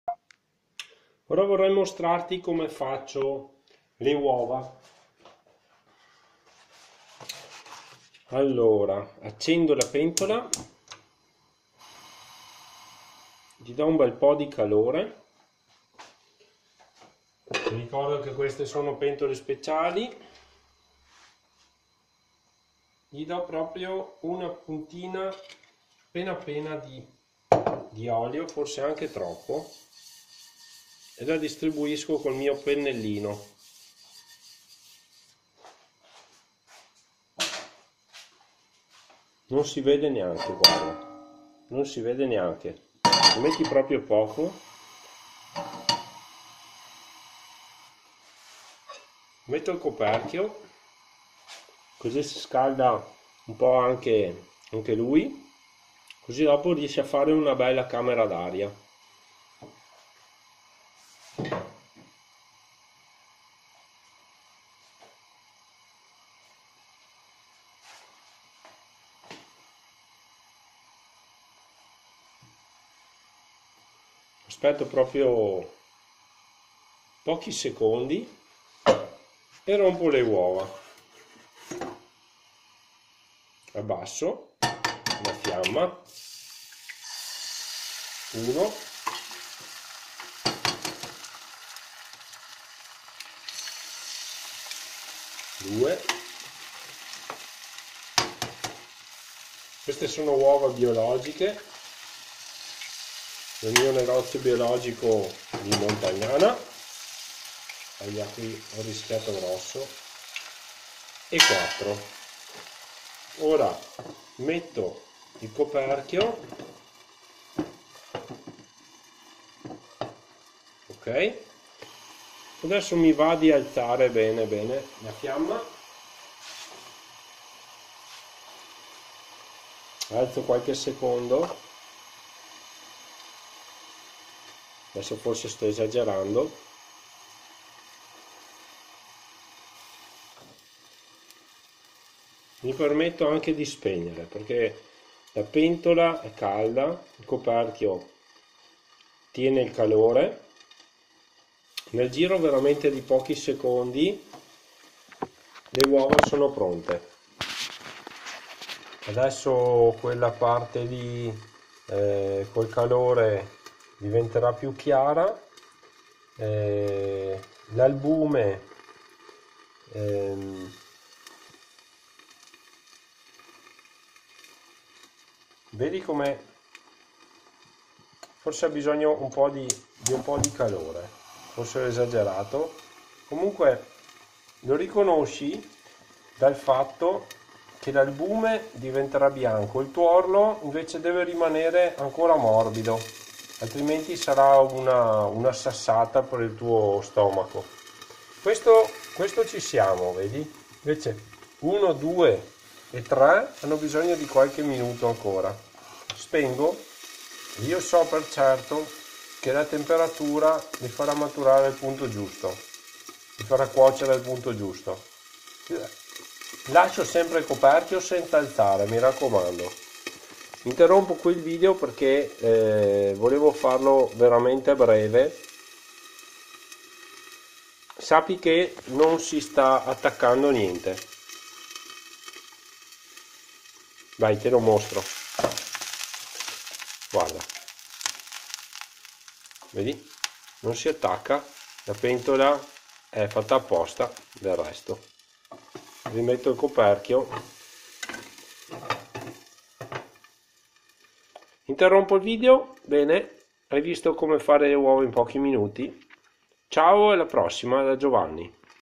Come preparare delle uova biologiche in padella antiaderente, mantenendo il tuorlo morbido
In questo video ti mostro come preparo, in poche mosse, delle uova biologiche in una padella antiaderente (senza Pfoa), mantenendo il tuorlo morbido

ciao, Giovanni

=== L' E-COMMERCE ===